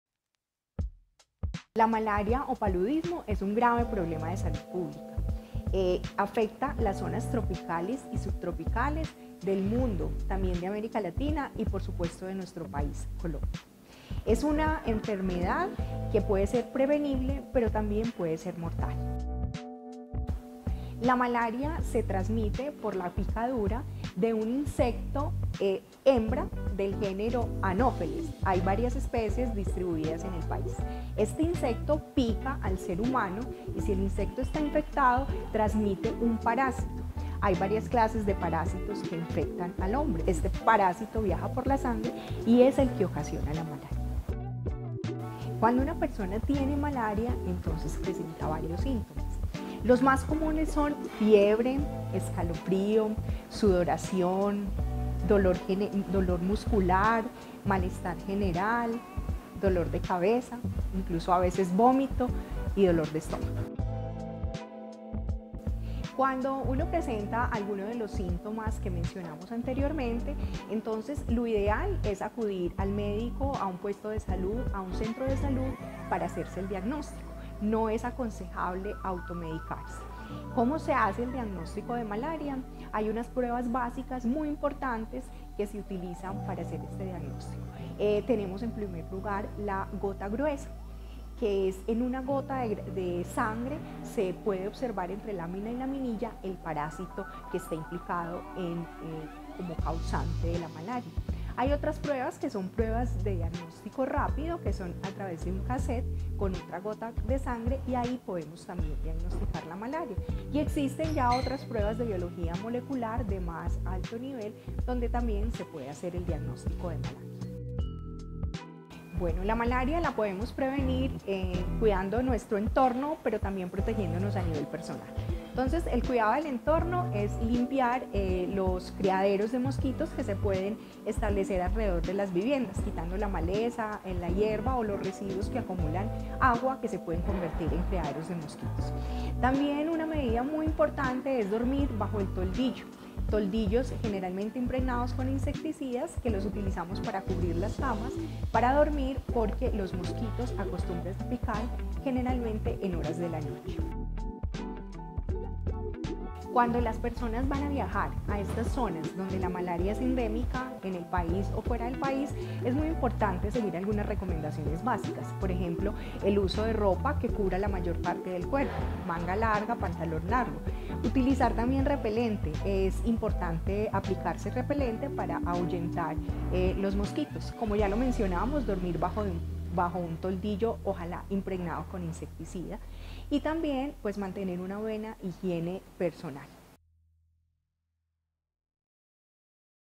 El paludismo o Malaria
El paludismo o Malaria es una infección parasitaria transmitida por mosquitos anofelinos. El 85% de Colombia esta situada por debajo de los 1.600 mts sobre el nivel del mar siendo las condiciones ambientales y demográficas  aptas para que se propague la Malaría. Hasta el mes de abril de 2024  se han reportado más de  36.625 personas  de contagios en los de Chocó (43 %); Antioquia (12,3 %); Córdoba (12 %); Risaralda (7,5 %); Nariño (7,3 %); Vaupés (4,9 %); Guainía (2,8 %); Buenaventura (2,4 %) y Cauca (2,1 %).